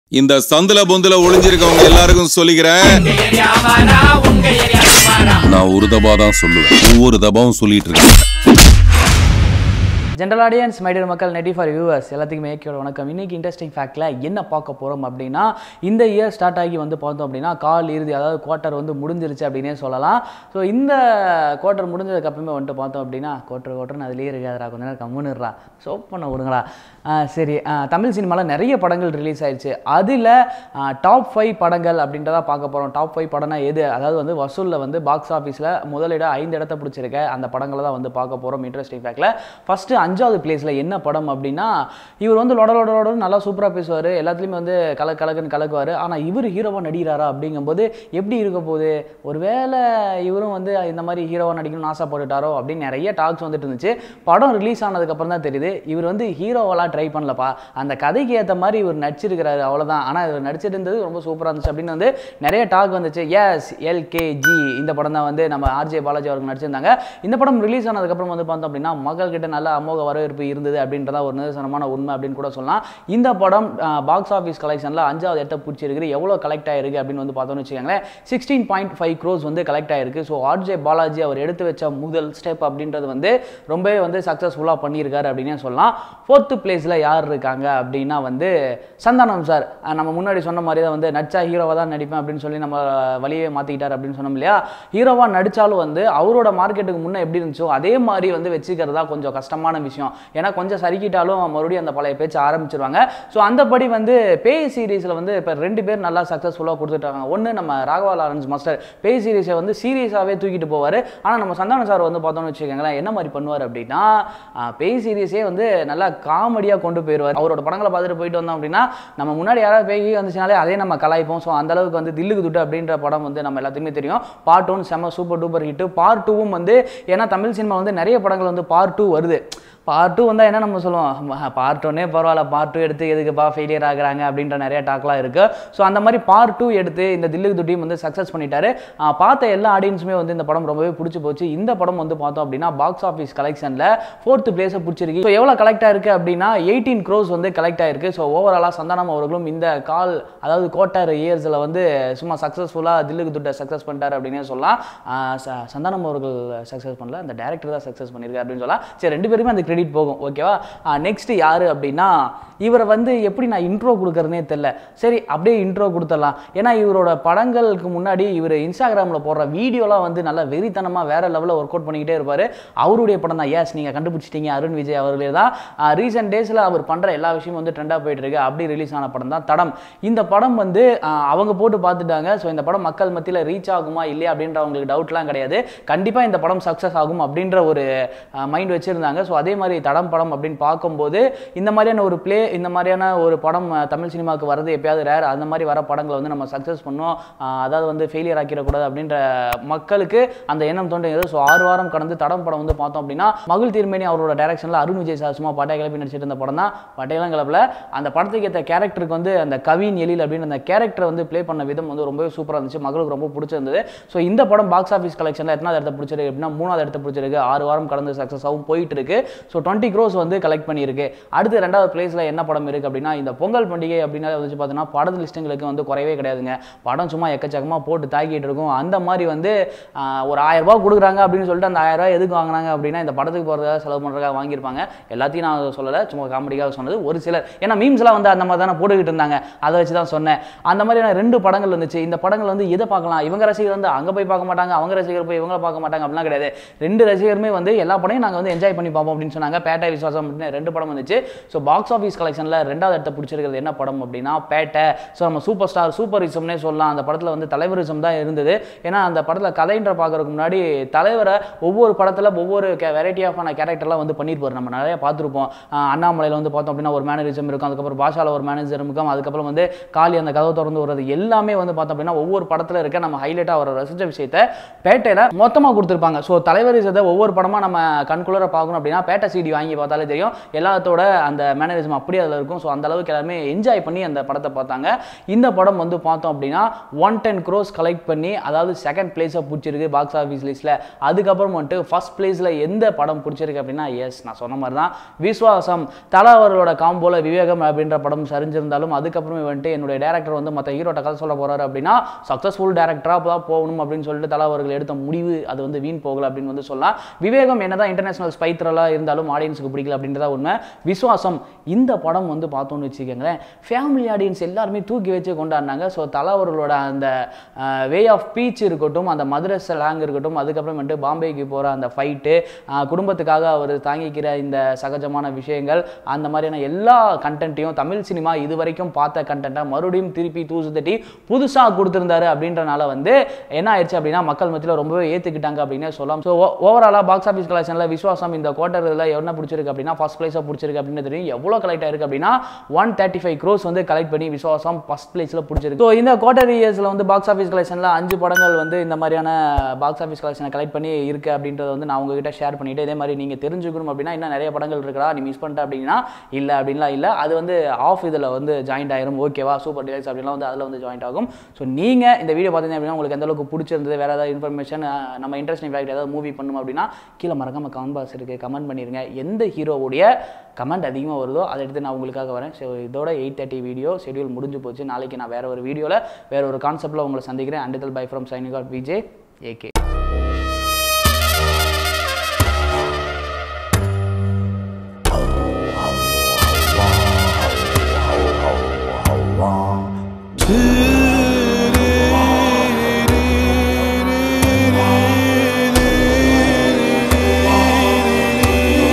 Box Office Collection Report | Who Is First Place | Interesting Facts AKReview | EP 58
Interesting Facts! Top 5 Box Office Collection In 2019! LKG is in the fifth position. LKG box office collection is Rs 16.5 crores. In the fourth place, we have Dhillukku Dhuddu 2. DD2 box office collection is Rs 18 crores. Thadam is in third place and it has collected Rs 20 crores at the box office. The second place goes to Superstar Rajinikanth movie Petta and it has collected Rs.110 crores. Do you know the movie that got the first place? Watch the video and get the complete box office collection reports!
Nettv4u a leading Cinema & Political news provider, find everything that happens around in Kollywood.